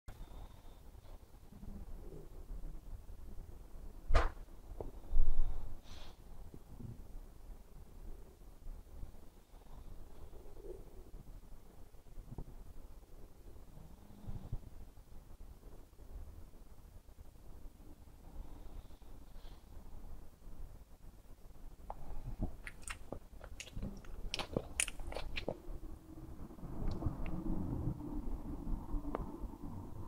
Pug lays on his side, staring off to nowhere (2023 - May 27)
Here's my pug just chilling out. He's laying on his side, on the sofa and just staring off into space. I always wonder what's going on in that little head of his. Is he thinking about something important? Solving pi? Or if he had a speech bubble over his head, would it just be empty. Only he knows!